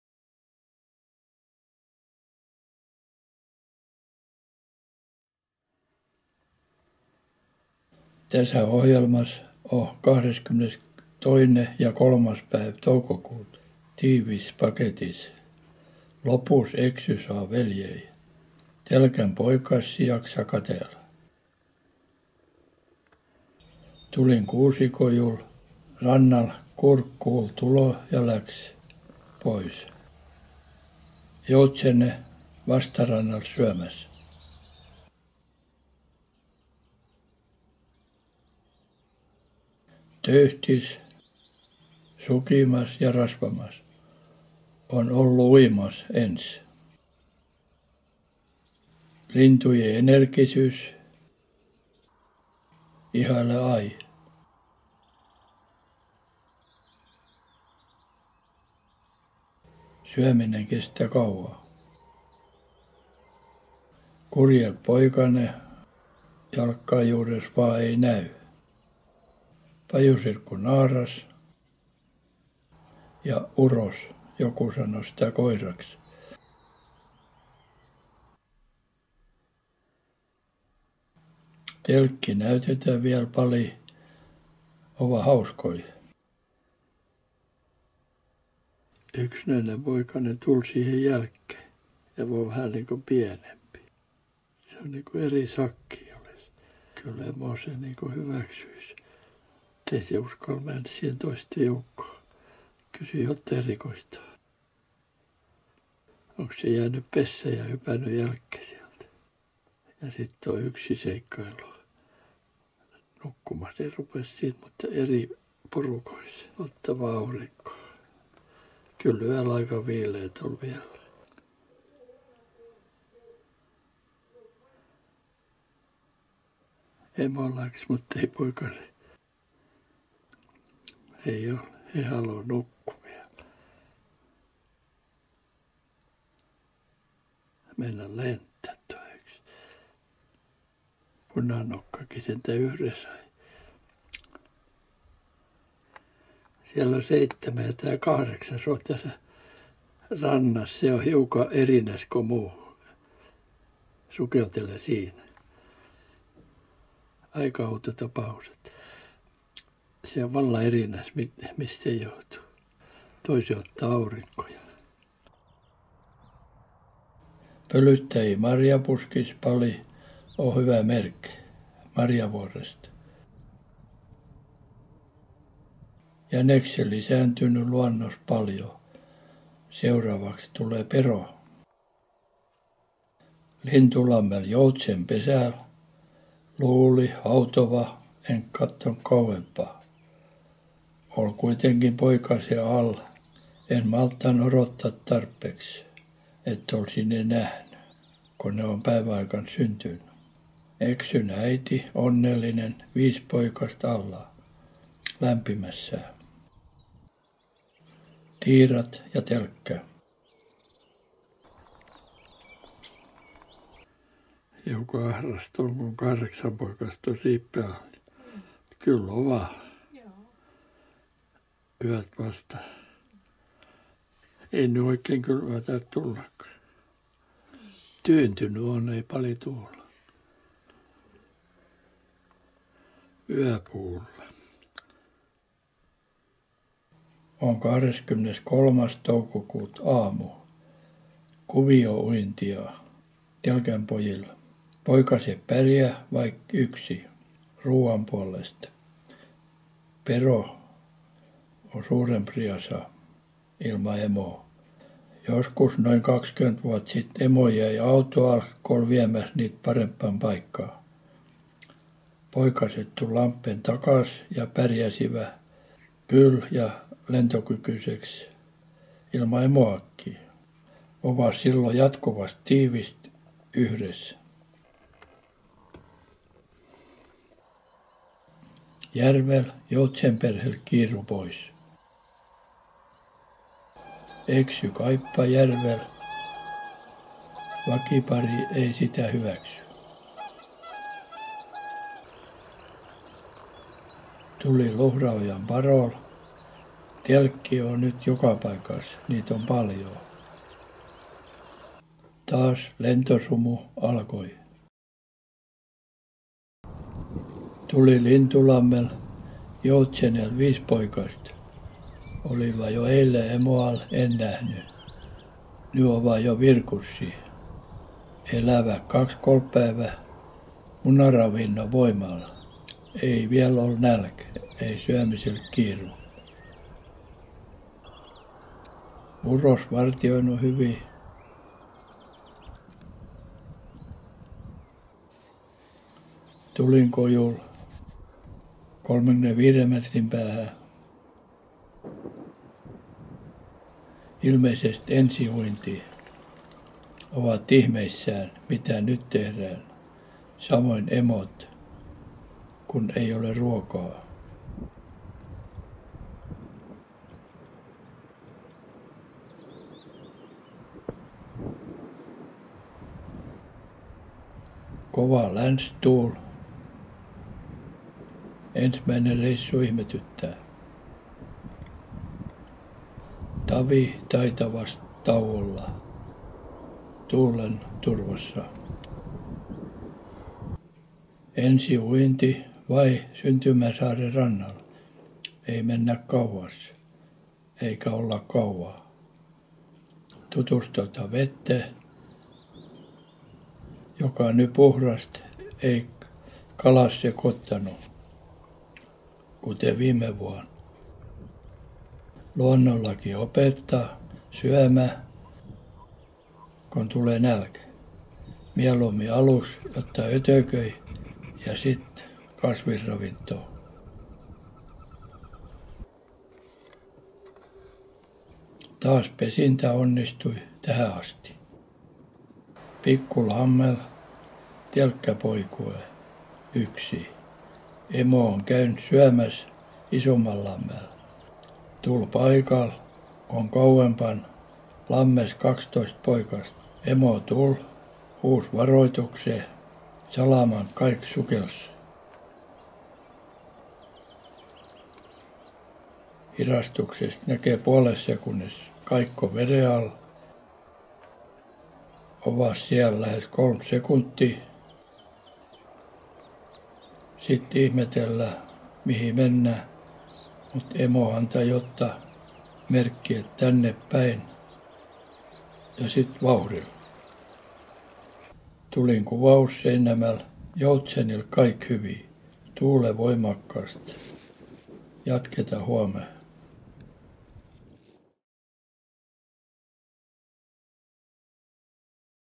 Luonto 22-23 5 yhteis-sukellus 12 poikast
Lintu lamme Joutsen saa 5 poikast